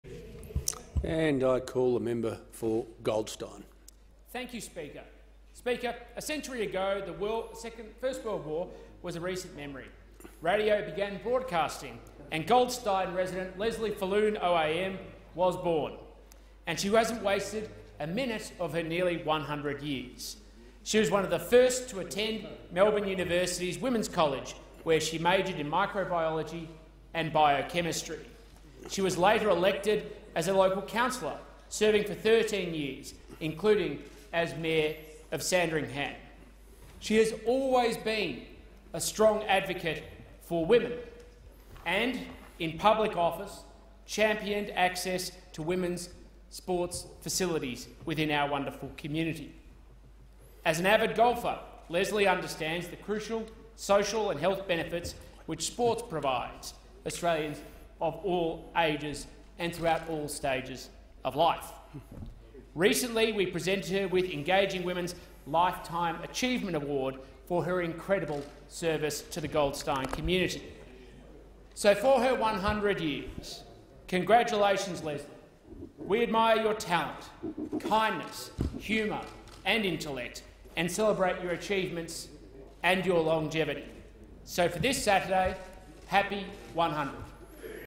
Tim Wilson MP | Lesley Falloon OAM 100th Birthday
"A century ago the First World War was a recent memory, radio began broadcasting and Goldstein resident Lesley Falloon OAM was born.

She hasn't wasted a minute of her nearly 100 years. She was one of the first to attend Melbourne University's Women's College where she majored in microbiology and biochemistry. She was later elected as a local councillor, serving for 13 years, including as Mayor of Sandringham.

She has always been a strong advocate for women and, in public office, championed access to women's sports facilities within our wonderful community. As an avid golfer, Lesley understands the crucial social and health benefits which sport provides to Australians of all ages and throughout all stages of life.

Recently, we presented her with Engaging Women's Lifetime Achievement Award for her incredible service to the Goldstein community. So, for your 100 years, congratulations, Lesley! We admire your talent, kindness, humour and intellect, and we celebrate your achievements and your longevity.

For this Saturday, happy 100th!"

Tim Wilson is the Federal Liberal Member for Goldstein.